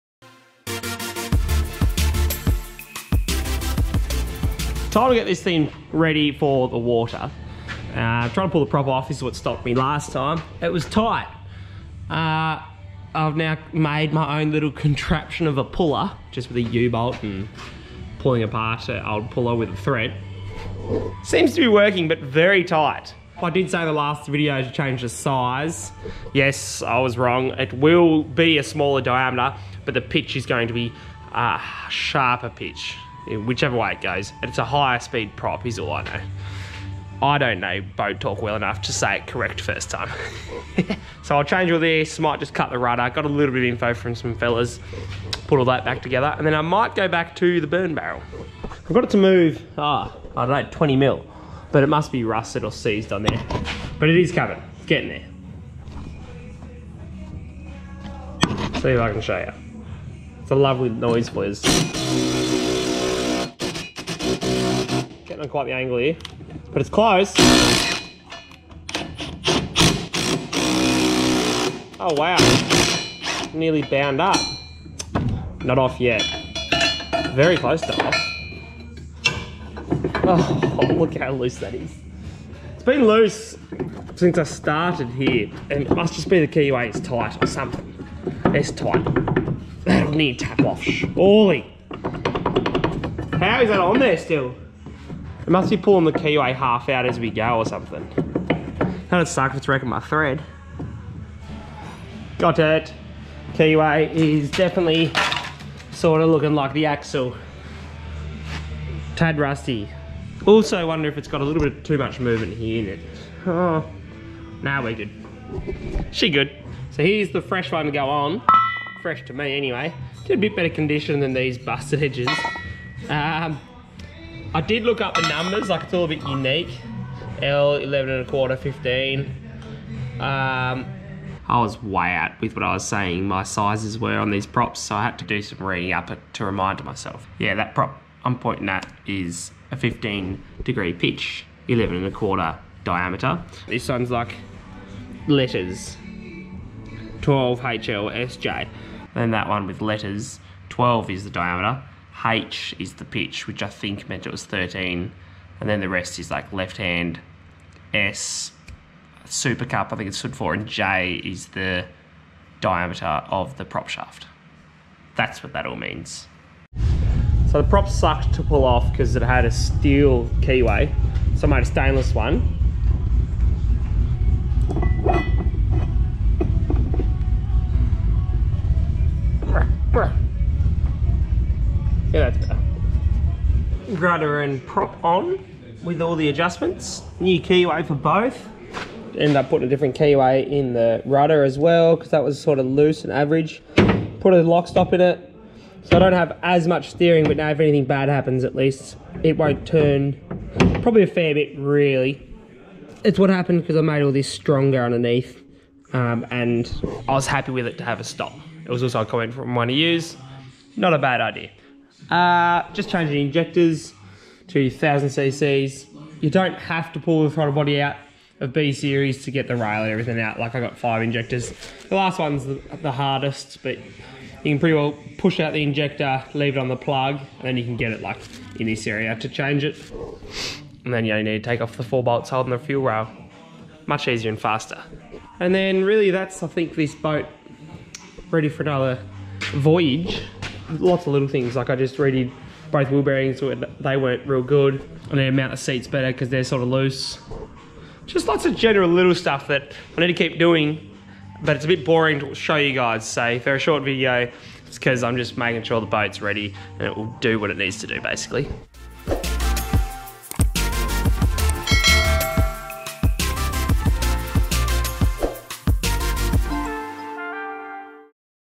New Prop for more Speed - Boat is ready to Scare me..
The prop and rudder were the final adjustments needed, before venturing back on the water to see how it responds! lets hope she loves it.

You really do help me make these videos happen, with the small money input and a huge Motivation booster!